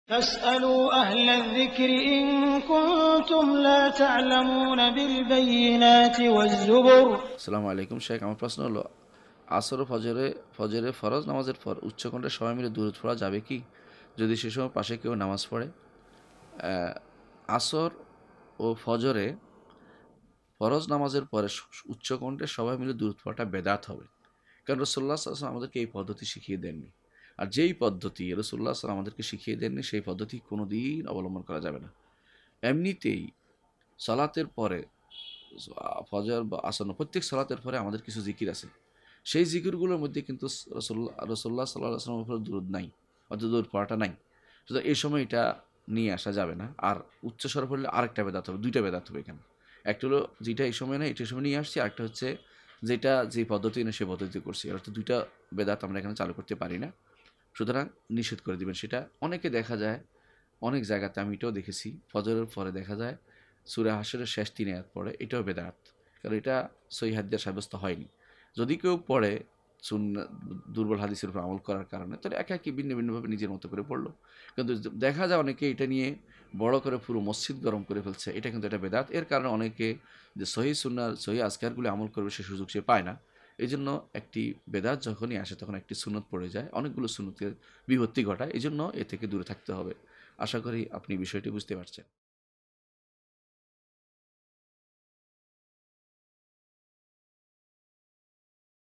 প্রশ্ন : ফজর ও আসরে দুরুদ পড়ার বিধান? শাইখ প্রফেসর ড. আবু বকর মুহাম্মাদ যাকারিয়া
প্রশ্ন : ফজর ও আসরে দুরুদ পড়ার বিধান?
শাইখ প্রফেসর ড. আবু বকর মুহাম্মাদ যাকারিয়া